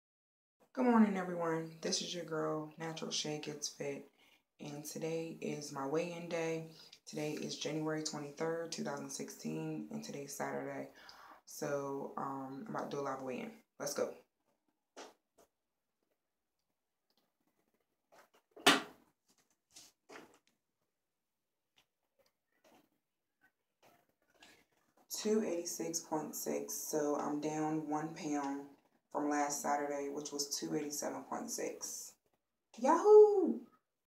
Weigh In Day! My weight loss journey of 2016
Today is weigh in day! We are getting a snow storm here. I am inside for the weekend. Anyways now let's get to this weigh in.
Hw: 310lbs
Starting weight since 1/1/16: 294.6lbs
Last weeks weigh in was 287.6
Current weigh in:??? Watch the video
Goal weight:220lbs???

Mfp: shayrizzy